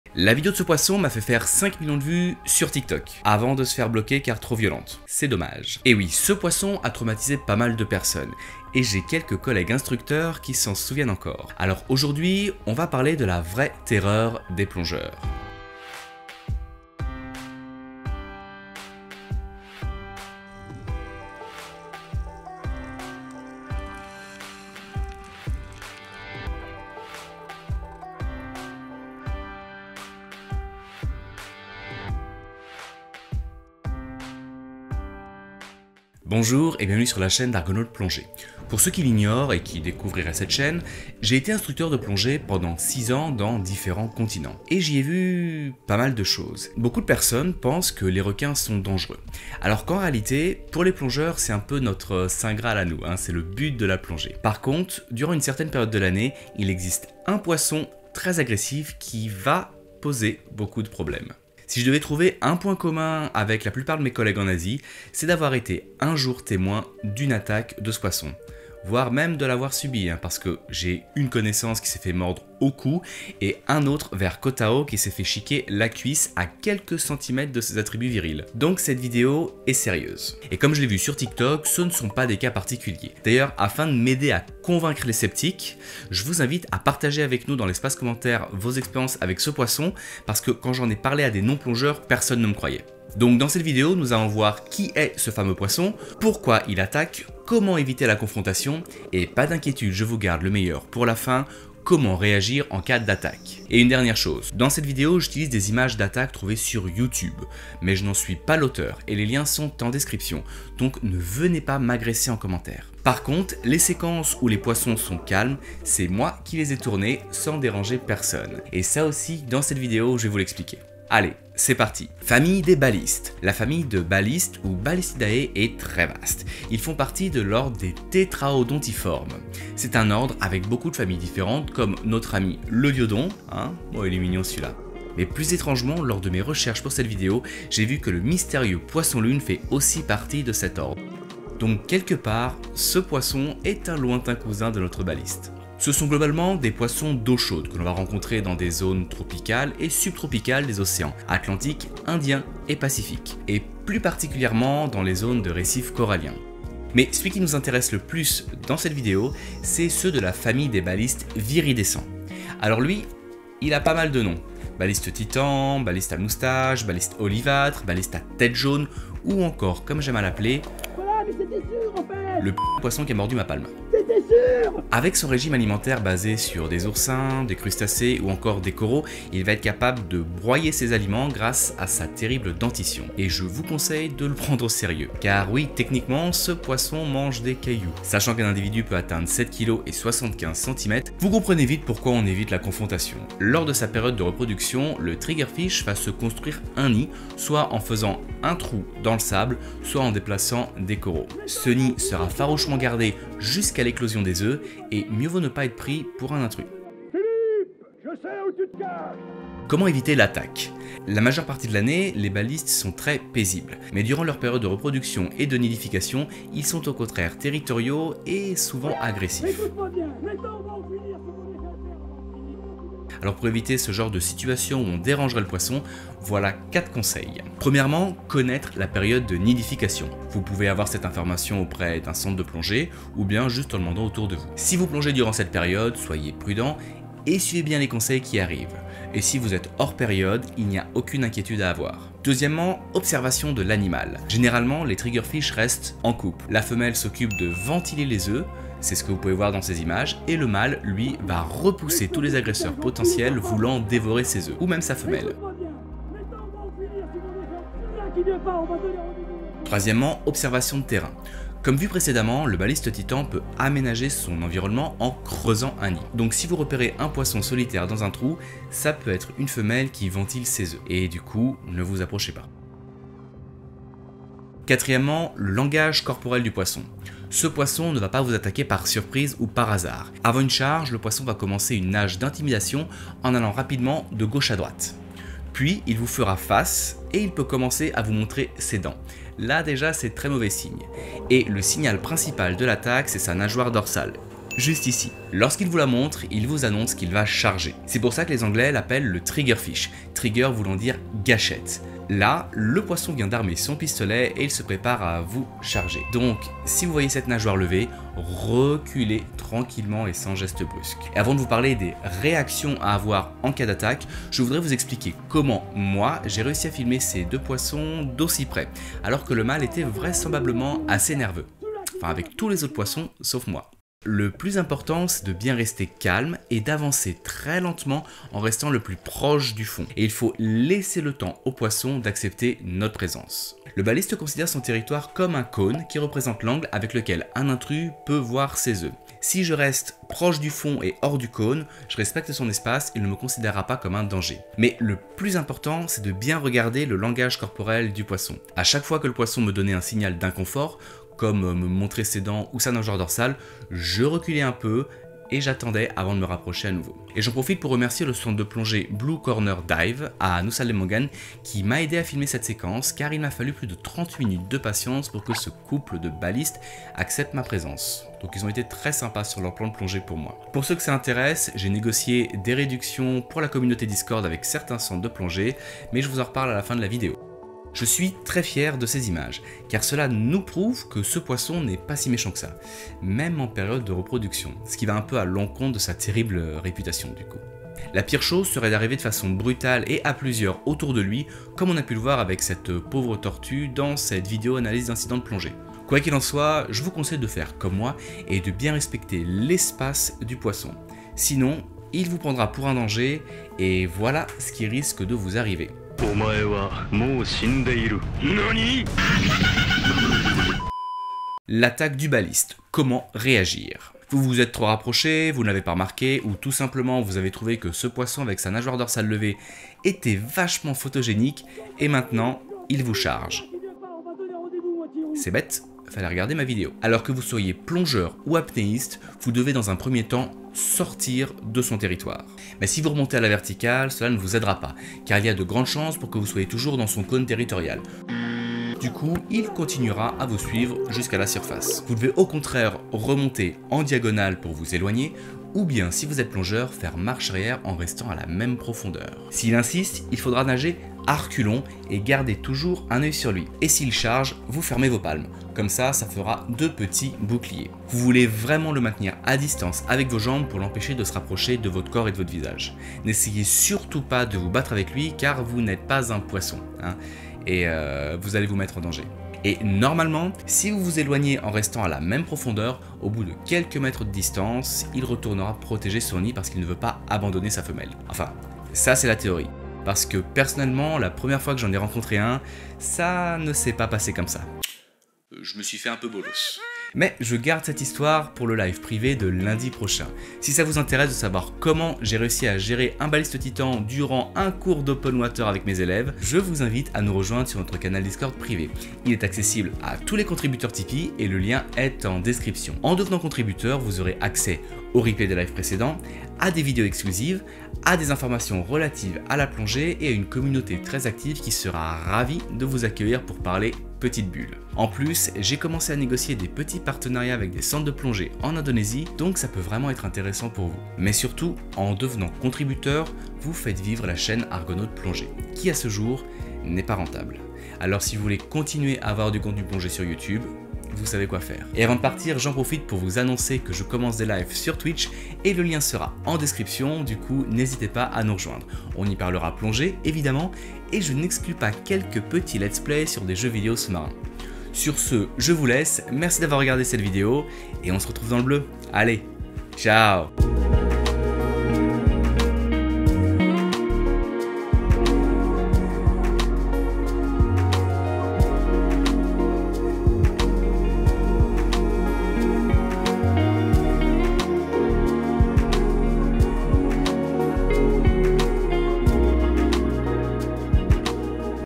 Baliste titan, comment plonger sans se faire mordre par ce poisson !?
Aujourd'hui je vais vous montrer mes techniques pour plonger avec un baliste titan sans ce faire mordre !  Car oui, ce poisson à des dents et il peut s'en service lors de vos plongées sous marine...

Le Balist titan ce retrouve dans la plupart des site de plongée en eau chaude ! Plus particulièrement dans les zone avec des récifs de corail.
Mer rouge, océan indien, pacifique, il est un peu partout et si vous n'y prenez pas garde, il pourra poser des problèmes au plongeurs débutants !

En effet, durant une certaines période de l'année, ce poisson deviens très téritorial...

Rencontrer la terreur des récifs, le Baliste titan !

Merci a Blue Corner dive pour m'avoir inviter sur leur bateau !

OLYMPUS TG6
CAISSON OLYMPUS

PANASONIC GH5II
OBJECTIF Paznasic 12 -35MM
OLYMPUS MACRO 30MM
OLYMPUS OBJECTIF pancake 12-35

BRAS /PINCE

00:00 Introduction
02:02 Genre et famille des poissons Balistes
03:29 Comment éviter l'attaque du Baliste Titan
05:25 Ma technique d'approche du Baliste
06:52 Comment gérer l'attaque du Baliste Olivâtre
08:06 Annonce spécial